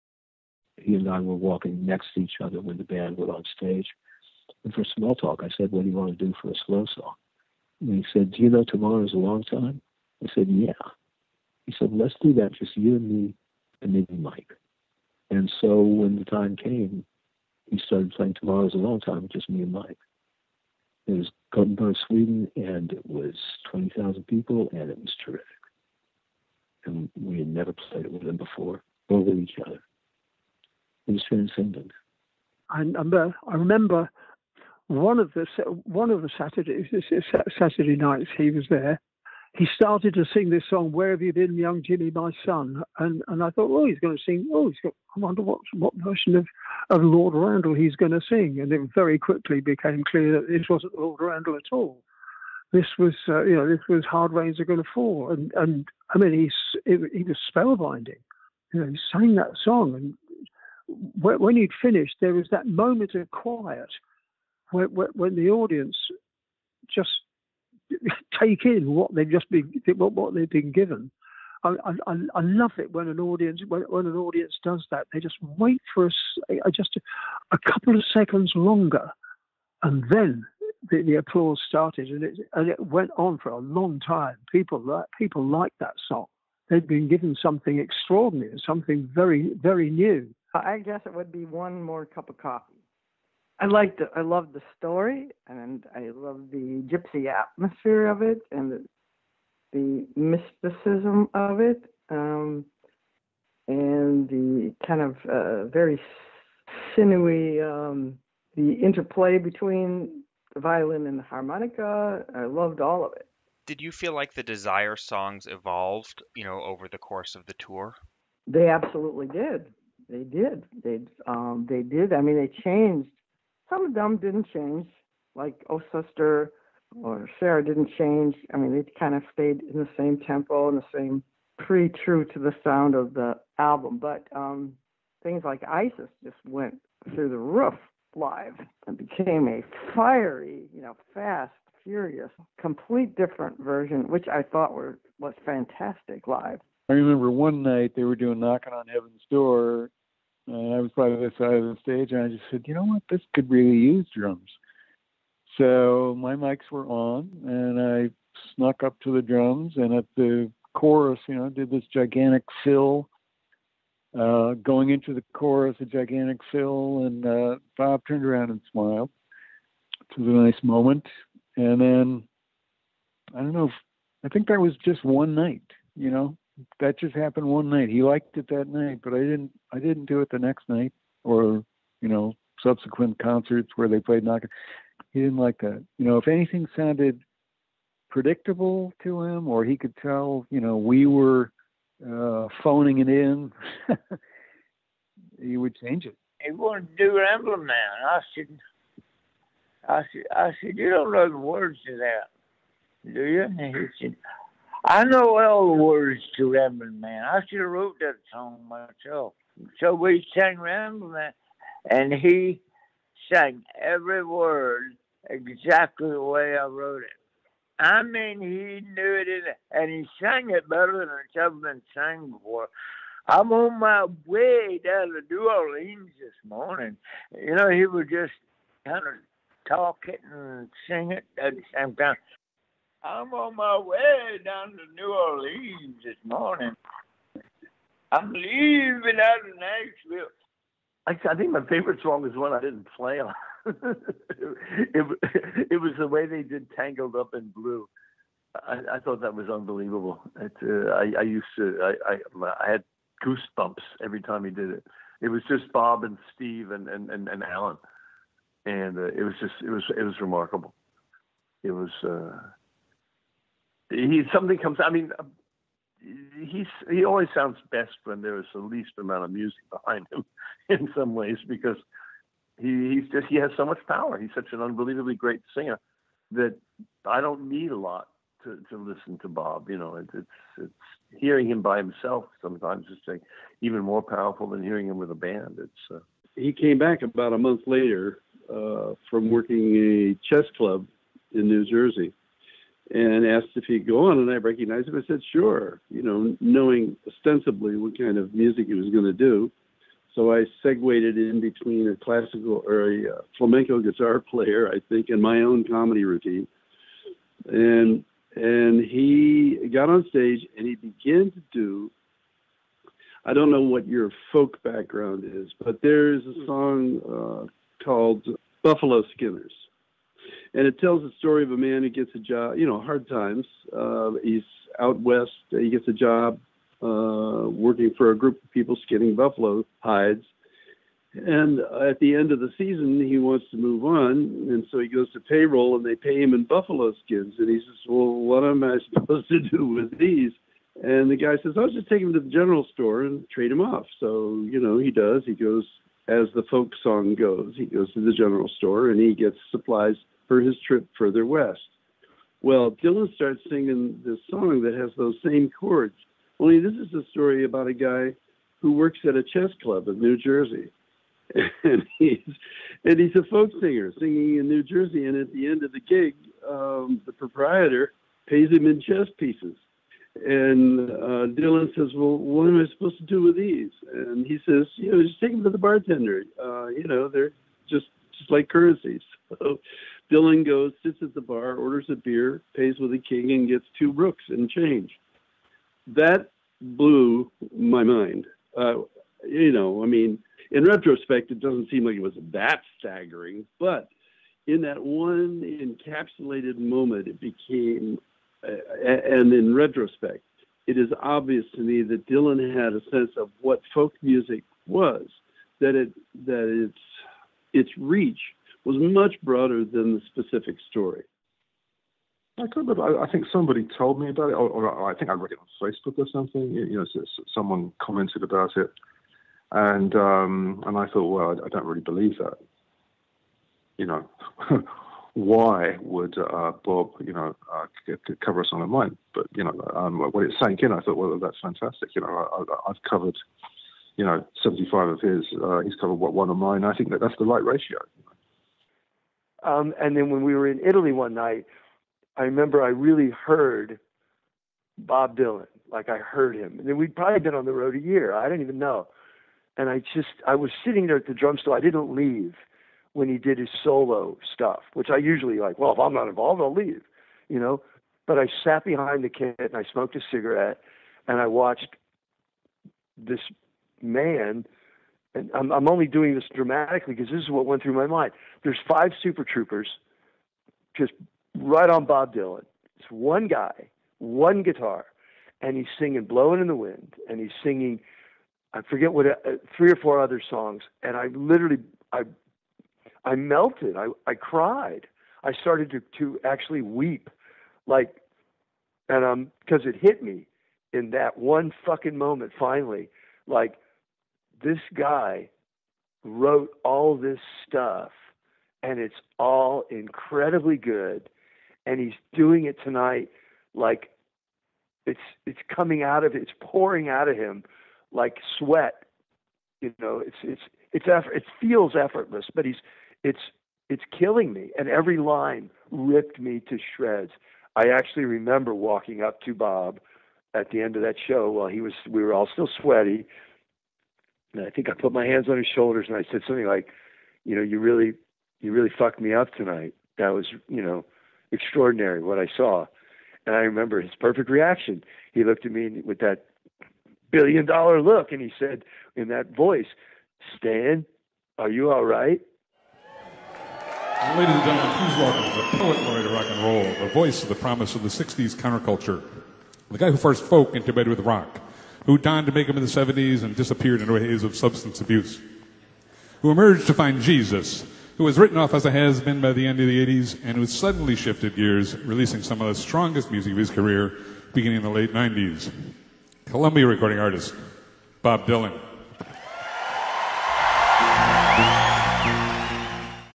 "The Most Memorable Song I Played with Bob Dylan"
Pledging My Time: Conversations with Bob Dylan Band Members out now!

Speakers in this video:
* Benmont Tench: Tom Petty & The Heartbreakers keyboardist
* Martin Carthy: British folk singer
* Scarlet Rivera: Rolling Thunder violinist
* Christopher Parker: First Never Ending Tour drummer
* Dickey Betts: Allman Brother who's sat in with Dylan
* Billy Cross: 1978 tour guitarist
* Noel Paul Stookey: Peter, Paul, & Mary
* Richard Thompson: Singer-songwriter who's played and toured with Dylan
* Stan Lynch: Tom Petty & The Heartbreakers drummer